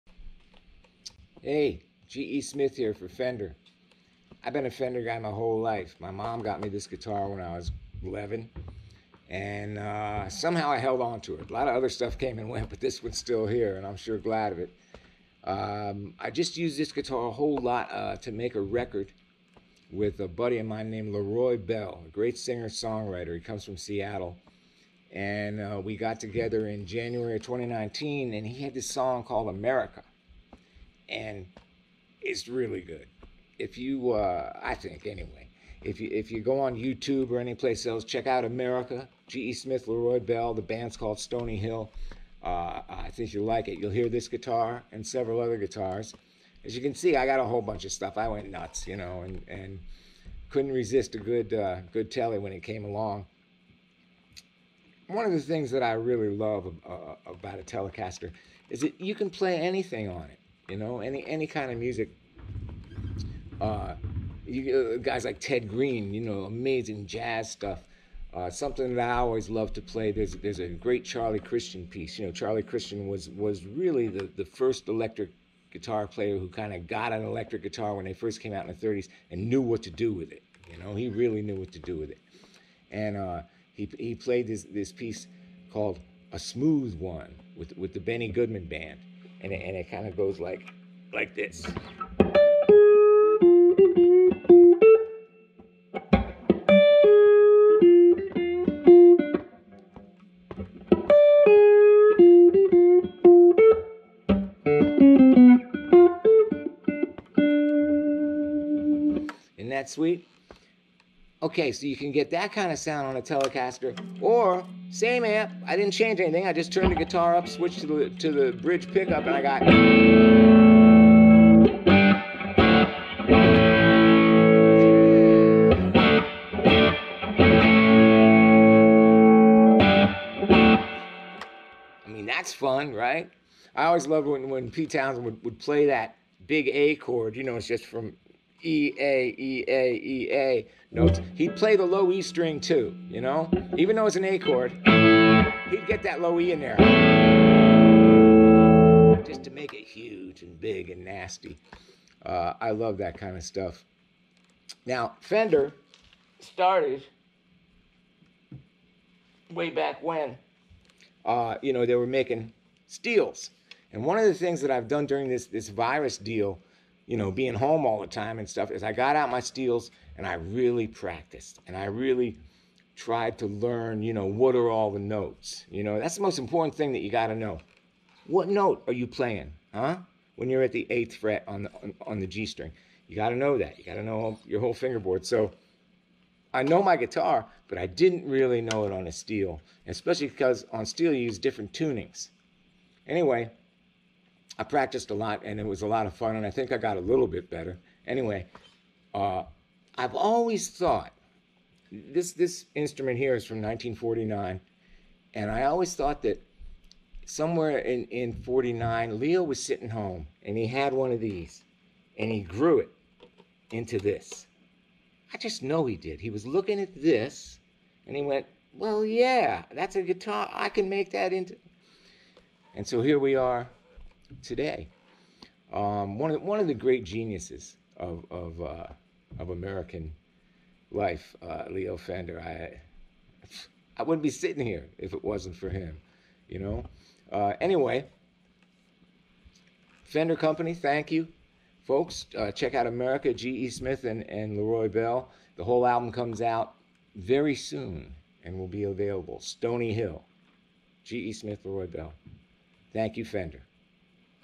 G.E. Smith On Vintage Gear, Telecaster Timeline & Fender Origins | Fender Artist Check-Ins
Fender Custom Shop signature artist G.E. Smith is perhaps best known for his Emmy-winning decade as the distinctively ponytailed musical director for Saturday Night Live, six years of tours with Hall & Oates, NET (never ending tour) with Bob Dylan and Rogers Waters, and gigs with David Bowie, Mick Jagger, and Tina Turner among many others. Smith checks in with a variety of vintage Fender gear to talk about his latest project as well as his own connection to Fender throughout his life.

For every artist that contributes a check-in, Fender will make a donation to the NAACP Legal Defense Fund.